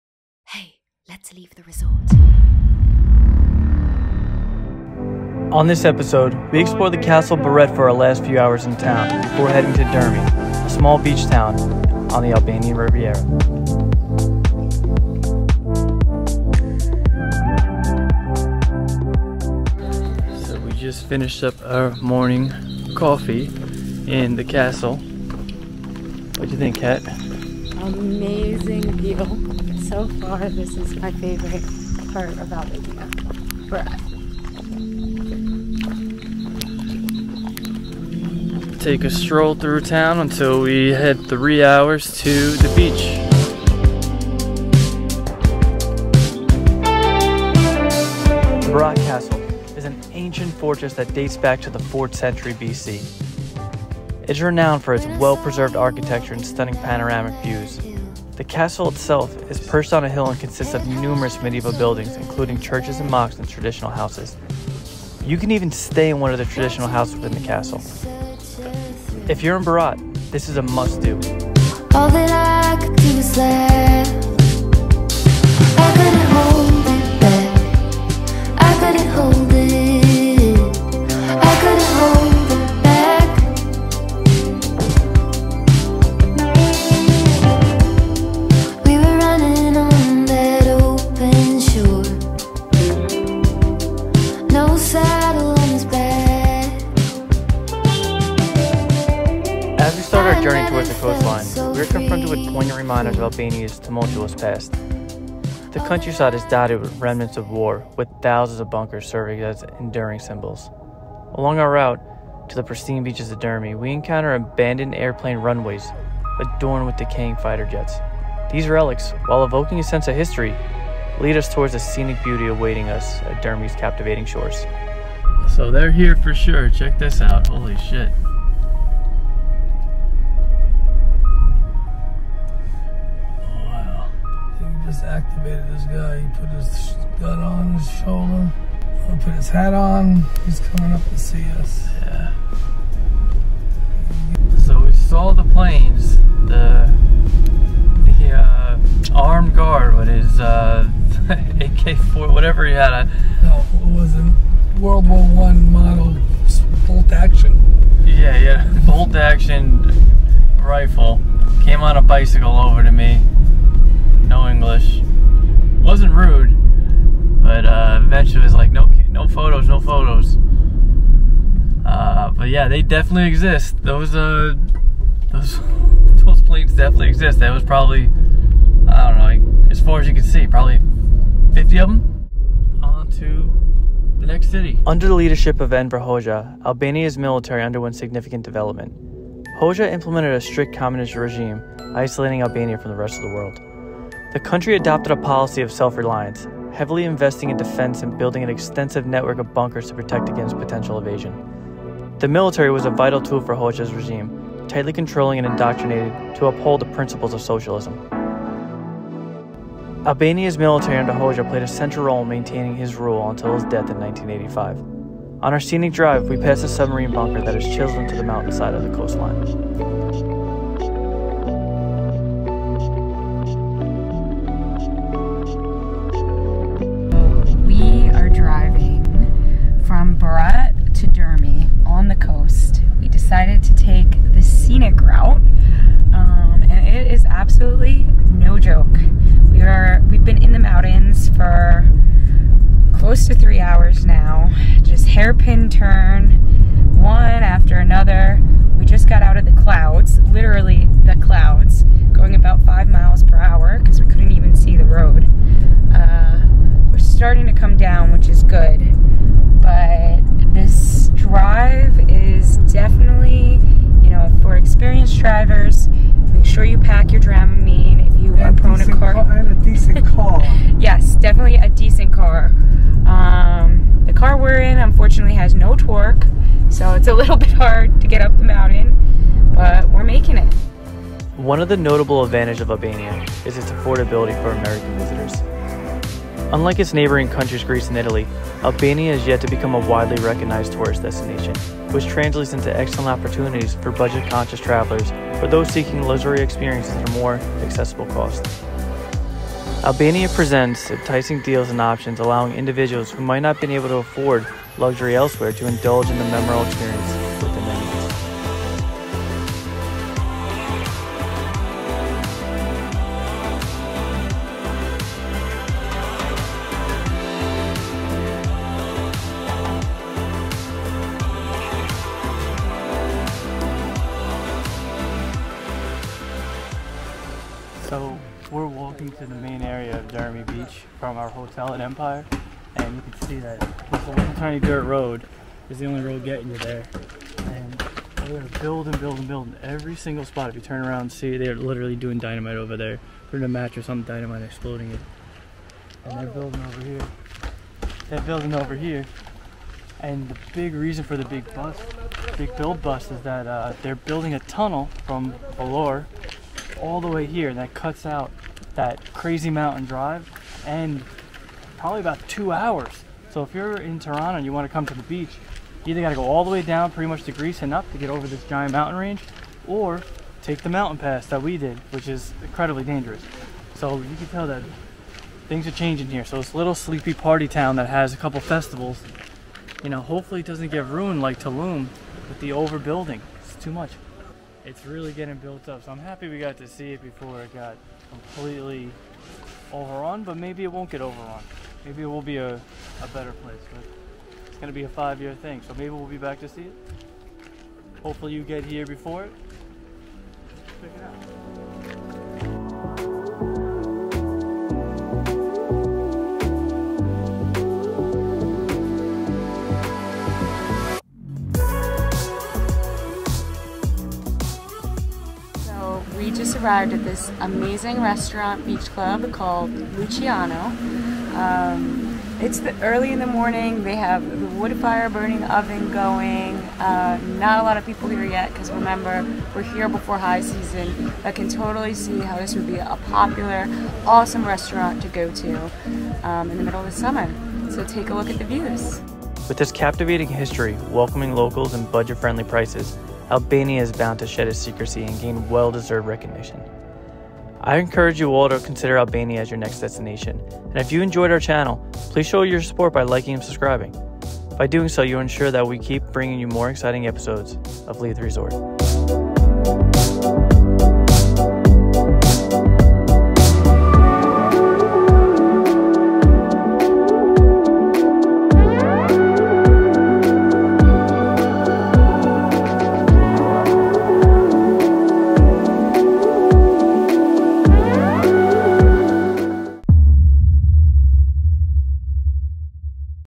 Should you road-trip Albania? Is it safe?!
We road-trip Albania from the mountainous town of Berat to the Albanian Rivera! Before our trip we explore the beautiful castle and then head to some of the top spots in Dhermi!

Let us know what you thought in the comments!

Socials: leavetheresort

0:05 castle to ourselves!
1:49 abandoned airfield
3:12 submarine bunker!
4:45 luxury beachfront rooms
7:15 best restaurant in Dhermi